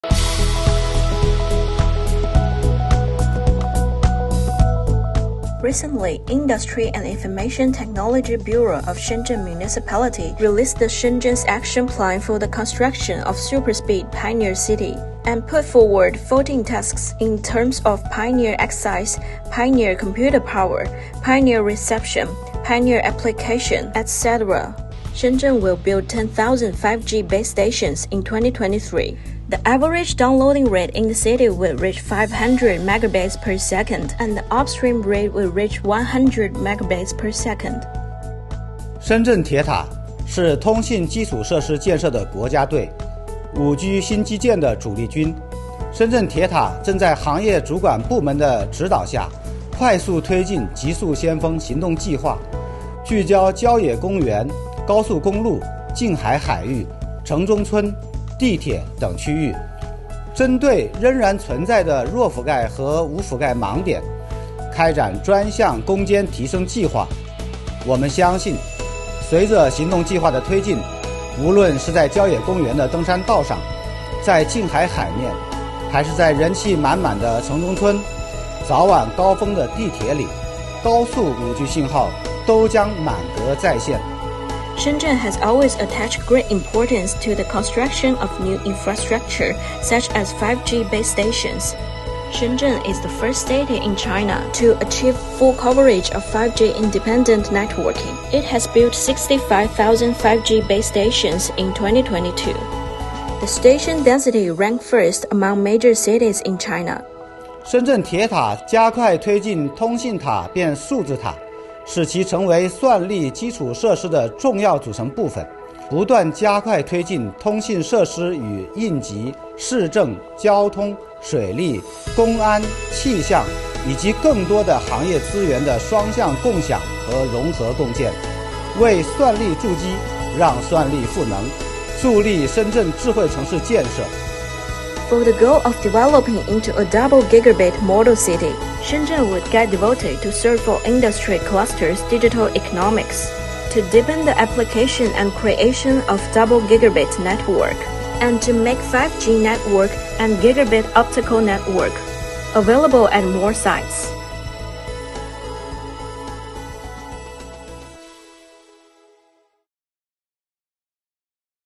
深圳推动极速先锋城市建设
【深圳推动极速先锋城市建设#】According to the “Shenzhen’s Action Plan for the Construction of Super-speed Pioneer City”,Shenzhen will build 10000 5G base stations in 2023. The company that builds 5G base stationsis rapidly promoting the “Action Plan” under the guidance of the industry authorities, to help build Shenzhen into a smart city#深圳2023年将新增一万个5G基站#